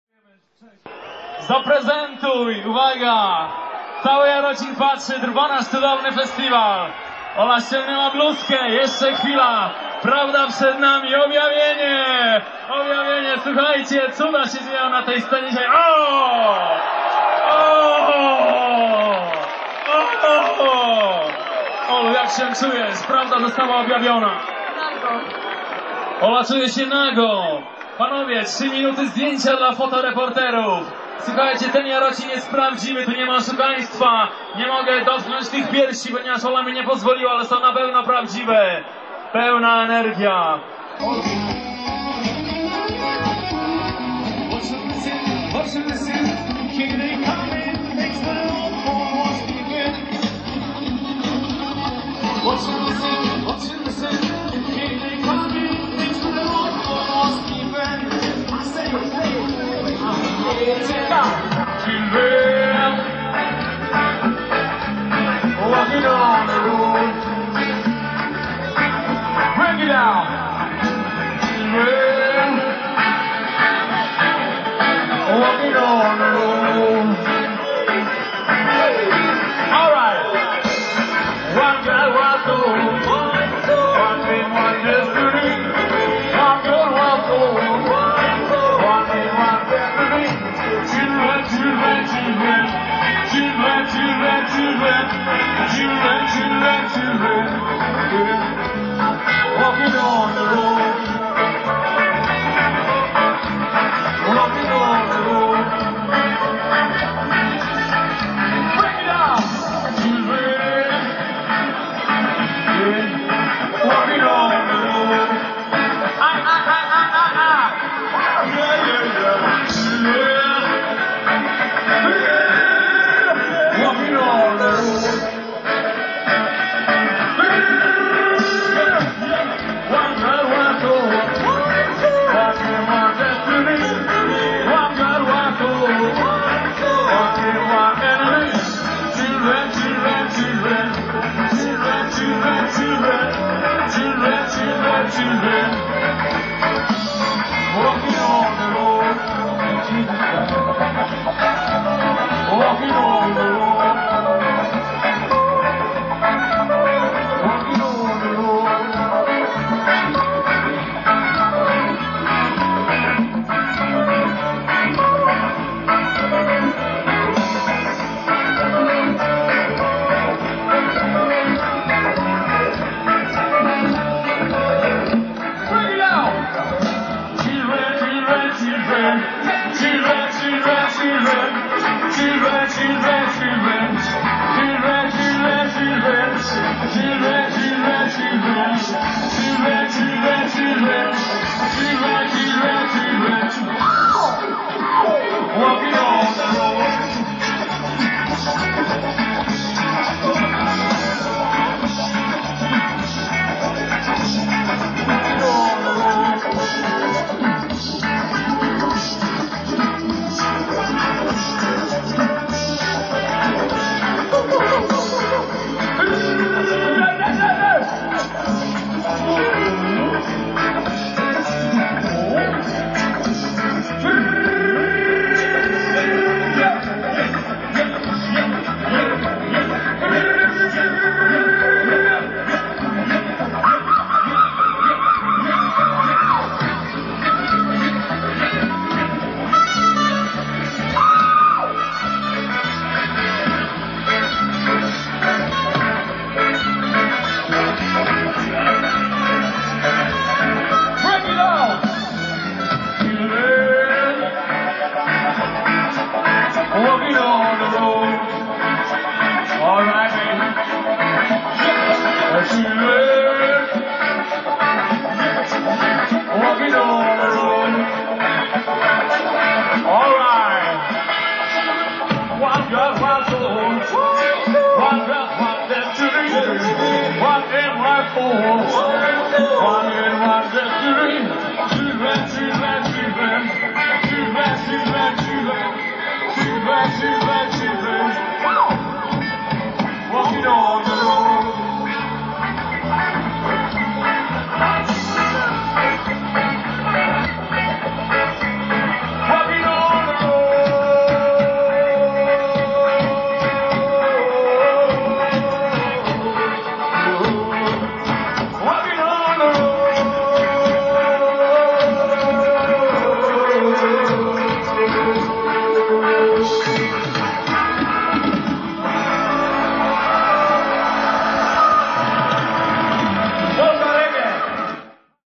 Jarocin 1993 Konkurs, Bakszysz, Izrael
Moje amatorskie nagrania z Jarocina